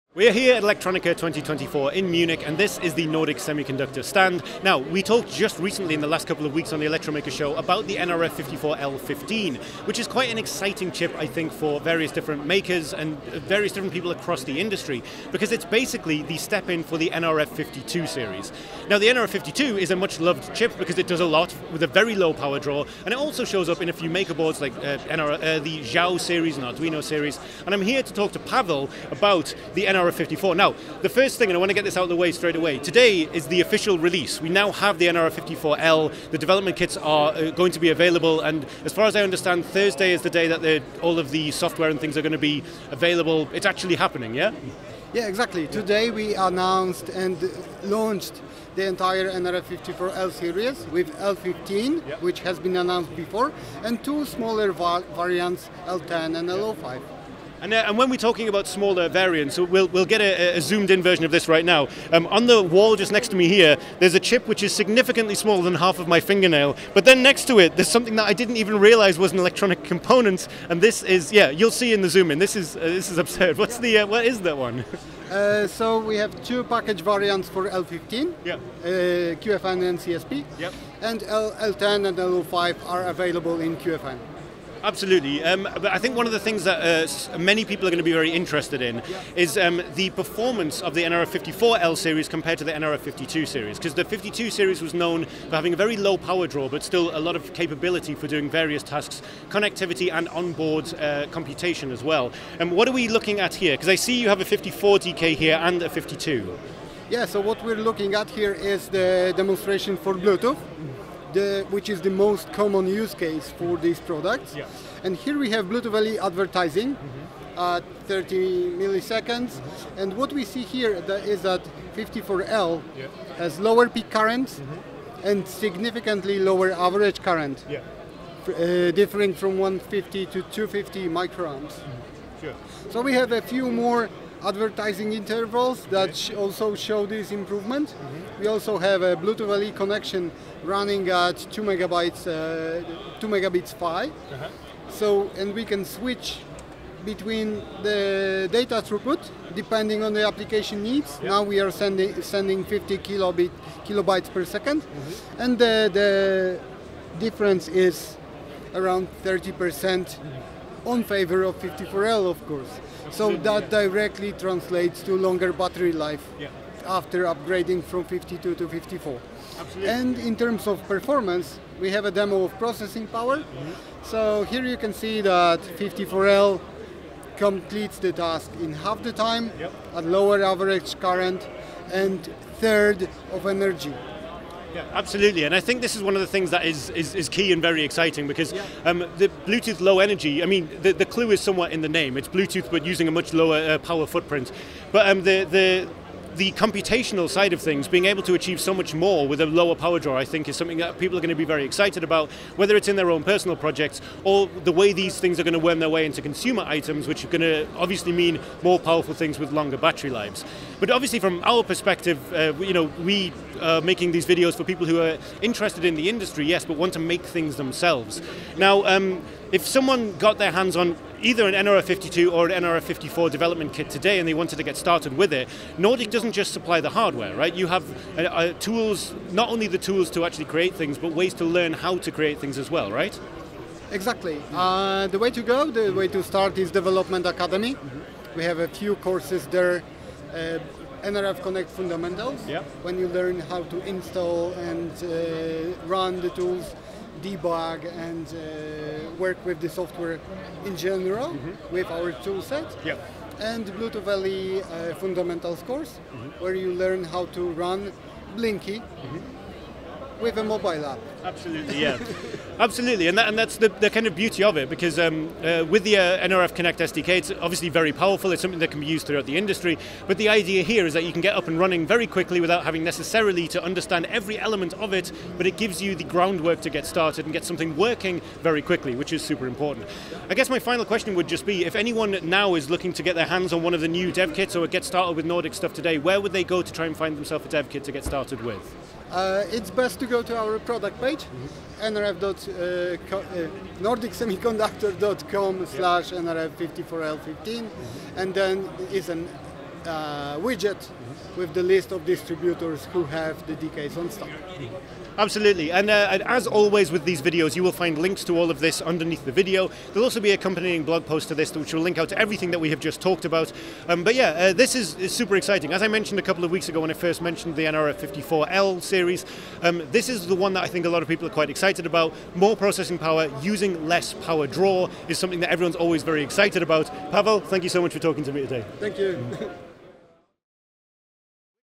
nRF54L vs nRF52 - What’s New in Nordic Semiconductor’s Latest Release?
At Electronica 2024 in Munich, Nordic Semiconductor unveiled its latest advancement in Bluetooth Low Energy technology, the nRF54L Series. This new lineup is set to build upon the popular nRF52 series, bringing significant improvements in performance and power efficiency, designed with both professional developers and DIY enthusiasts in mind. Recently highlighted on the Electromaker Show, the nRF54L Series promises to meet the needs of a broad range of applications, from IoT devices to consumer electronics, where long-lasting battery life and robust connectivity are essential.

0:00 - Overview of the NRF54L Series
0:19 - NRF52 vs. NRF54: What’s New and Improved
0:33 - Official Release of the NRF54L Series
0:57 - Two Package Variants: QFN and CSP
1:28 - Comparing Power Efficiency: NRF54L vs. NRF52
2:21 - Enhanced Bluetooth LE Performance
2:43 - Battery Life Improvements with the NRF54L
2:56 - Boosted Processing Power and Reduced Energy Consumption
3:52 - Getting Started with NRF Development Kits and Tools
4:22 - NRF Connect SDK: Fast and Easy Project Setup
4:46 - Where to Buy NRF54L Development Kits

▬ Support us! ▬▬▬▬▬▬▬▬▬▬